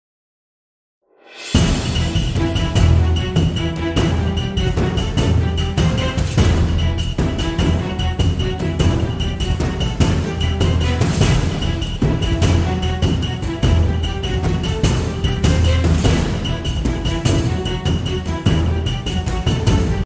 Uruguay im Zweiten Weltkrieg: Vom neutralen Hafen zur demokratischen Bastion
🕰️🌎 Vom Kriegsrefugium zur modernen Demokratie! Entdecke, wie der Zweite Weltkrieg Uruguay geformt hat – von einem neutralen Zufluchtsort bis hin zu einem Bollwerk der Demokratie und des Friedens.

🎥 In diesem Reel nehmen wir dich mit auf eine Reise durch die Zeit – von historischen Momenten bis hin zu den lebendigen Straßen des heutigen Uruguay. Erfahre, wie das Land seine dunkelsten Stunden meisterte und zu einem Symbol für Hoffnung und Fortschritt wurde.